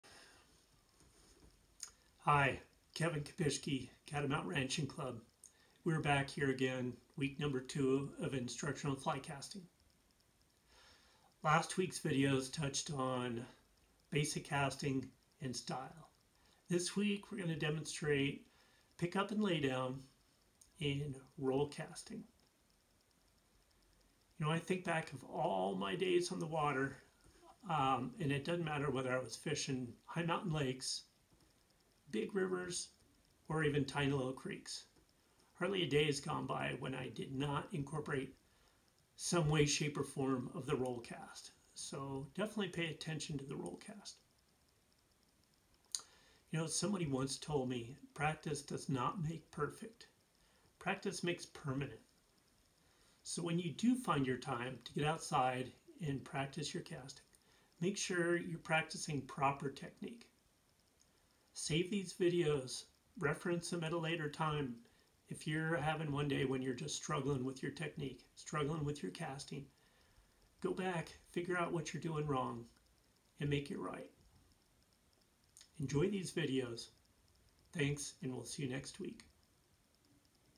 Kevin's Fly Fishing introduction to rollcasting
Don’t have the luxury of a full back cast? Use a roll cast.